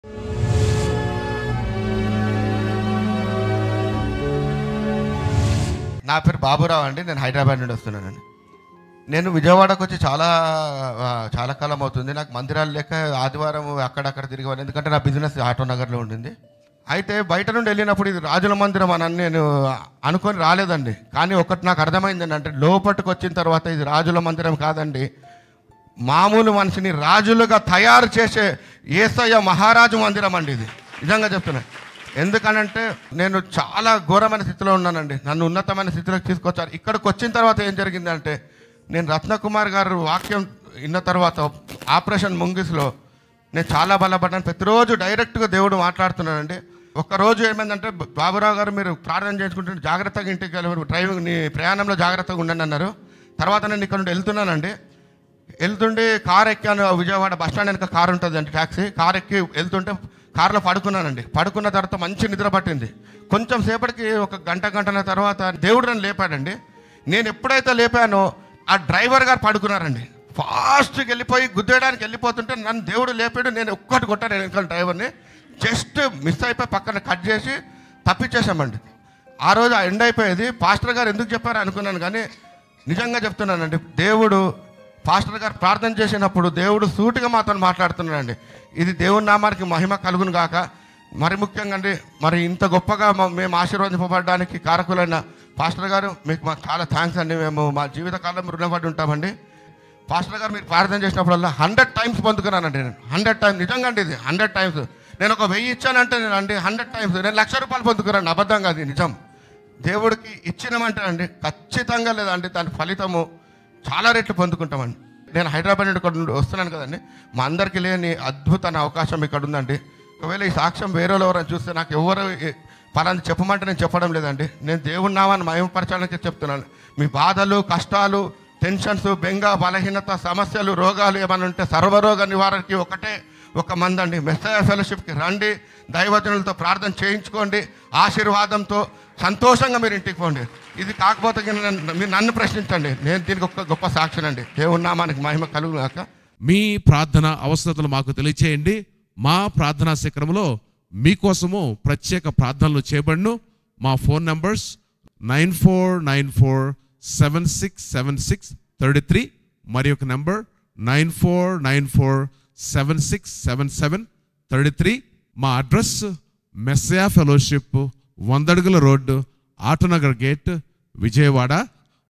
Testimony I. Babu Rao Garu from Hyderabad - ఐ.బాబు రావు గారి సాక్ష్యం
I  invite you to watch the testimony of I. Babu Rao Garu from Hyderabad. While driving through Autongar, he discovered Messiah Fellowship Church located on 100 Feet Road, Autonagar, Vijayawada. He has been part of our church for more than one year. Be blessed by watching his testimony.